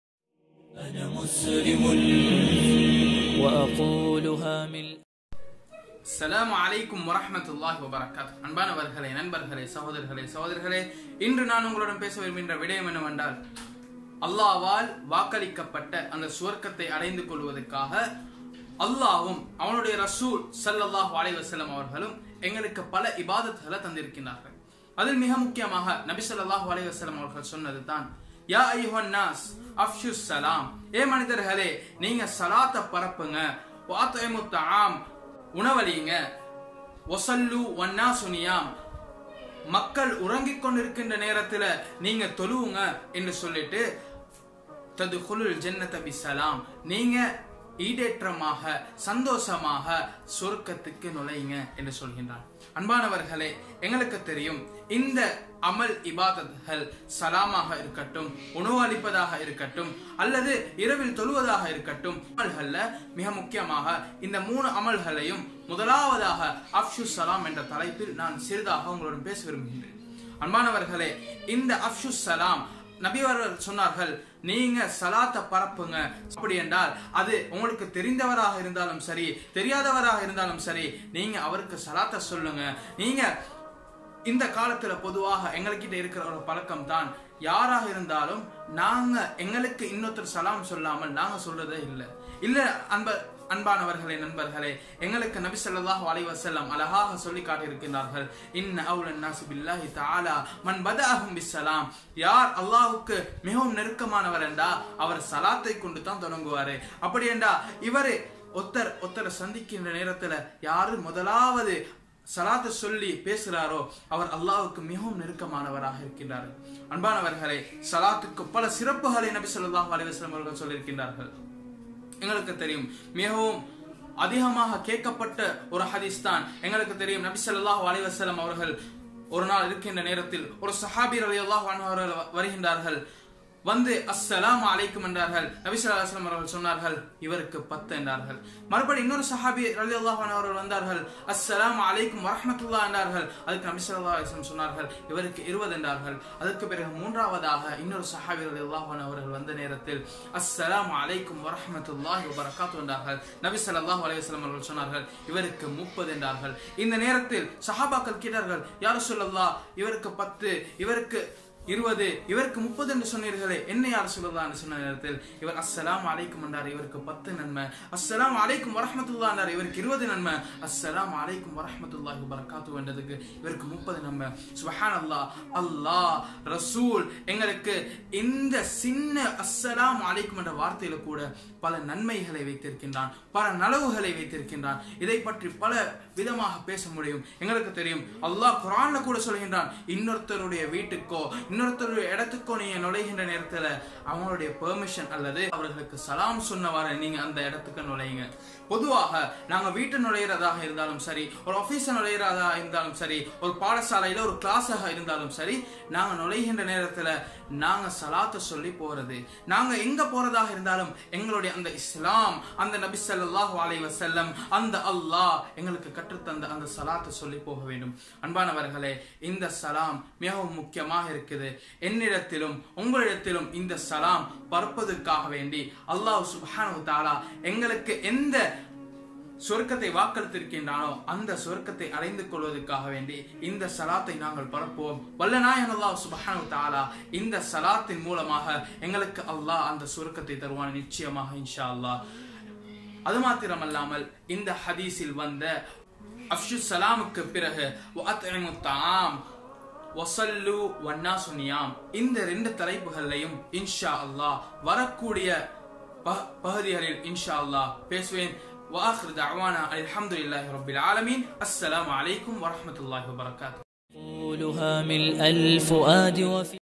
WAY TO JANNAH (சொர்க்கத்தின் வழி) (EPISODE 01)
WAY TO JANNAH is a series which has 3 Episodes based on a Hadeeth of
prophet Muhammed (صلى الله عليه وسلم)
Source of hadeeth :(Thirmizi)

Topics
Episode 01 : Spread the Salam
Episode 02 : Feed the Hungry
Episode 03 : Pray at Night when  people are sleeping

Next Episodes will be COMING SOON..

Episode 02